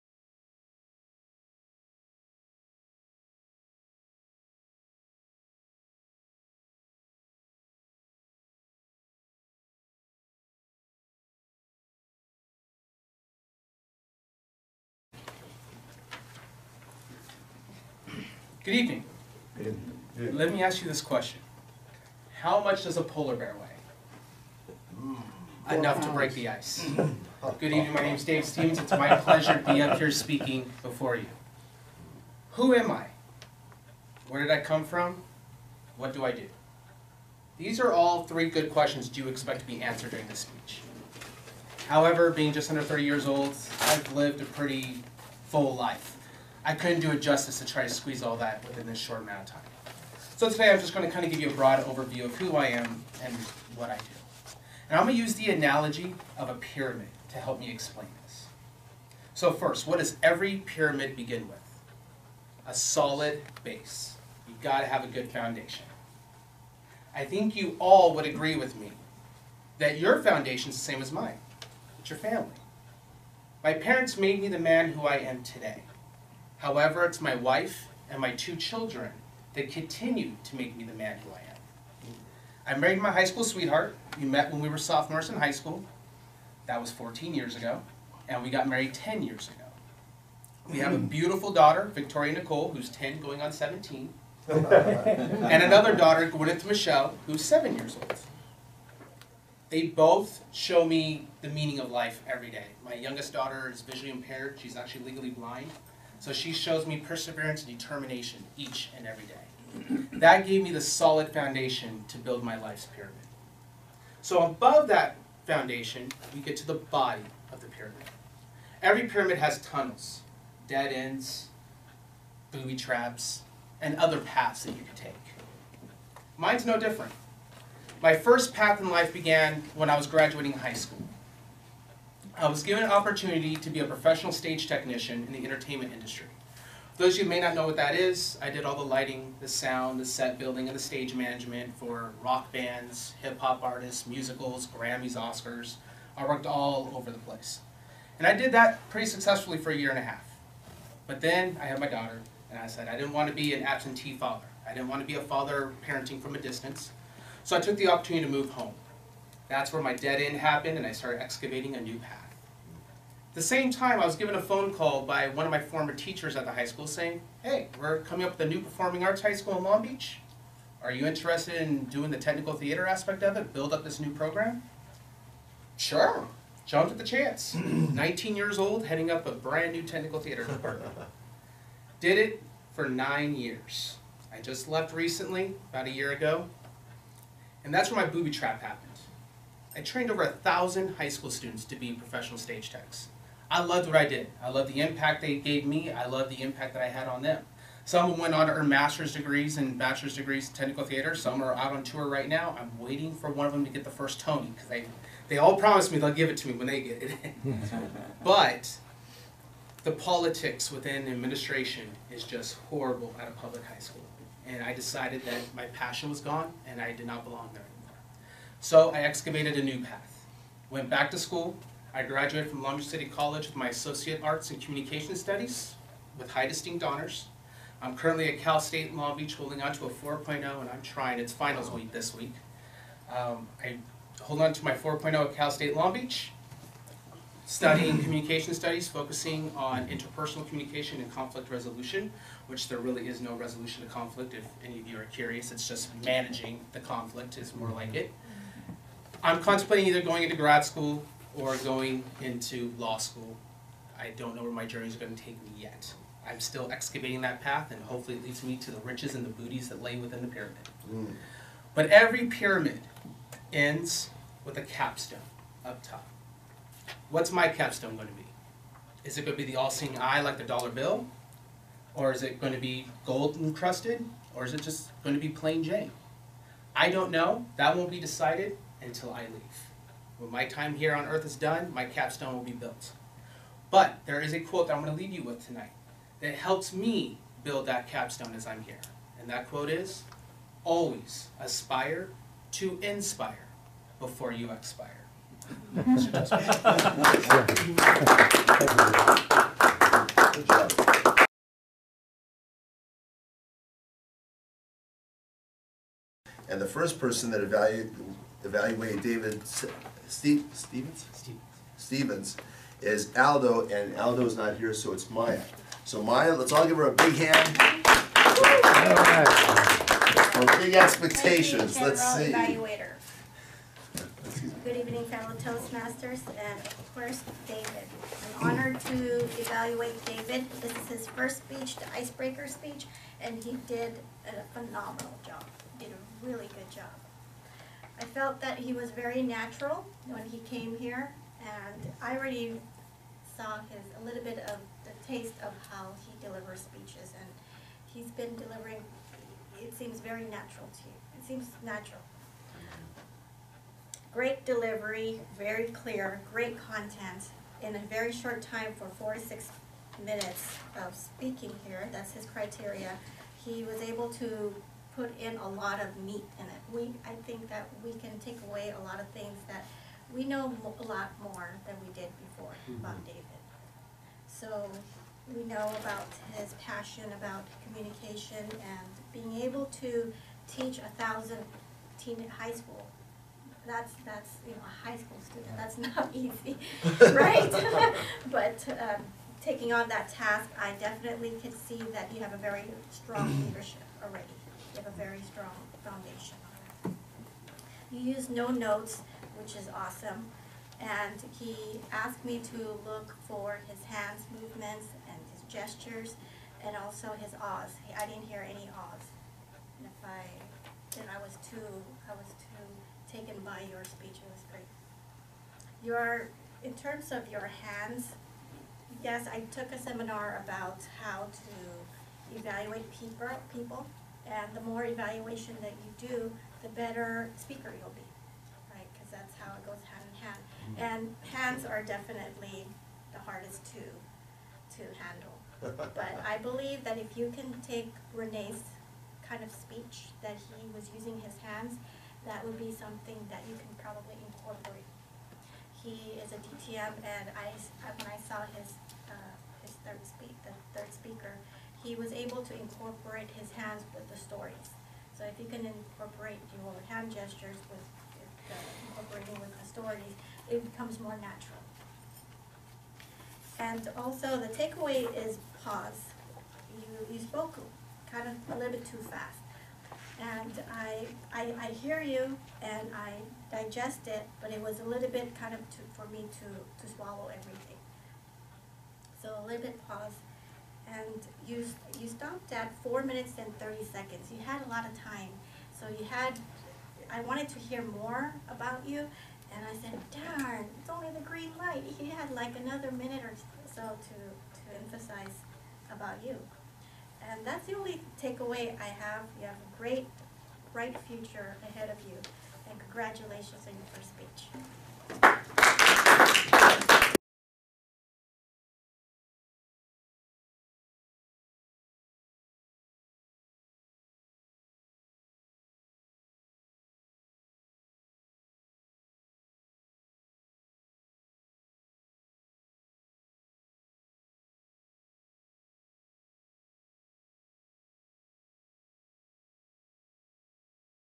David Stephens - Ice Breaker - May 13, 2014
David Stephens' first speech, the Ice Breaker.